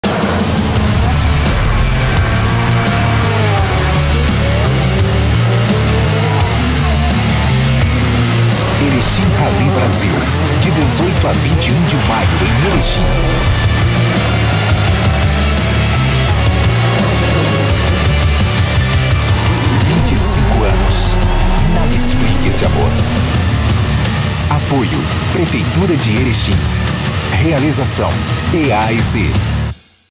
Erechim Rally Brasil - Comercial
Erechim Rally Brasil
25 anos de um relacionamento entre público e evento, ocitocina e adrenalina, perrengue e glória.
Onde a história premia o mais rápido com a emoção de ser campeão.
De 18 a 21 de Maio em Erechim - RS

Erechim Rally Brasil - nada explica esse amor.

Siga a gente no Instagram e no Twitter. @ErechimRallyBrasil

Imagens: Fernando Genro, Marcelo Pilli, Juliano Kalkmann, Vitor Sendra, Fabio Portella, Jean da Silveira, Rafael Gomes, Prime Comunicação e Rally BR.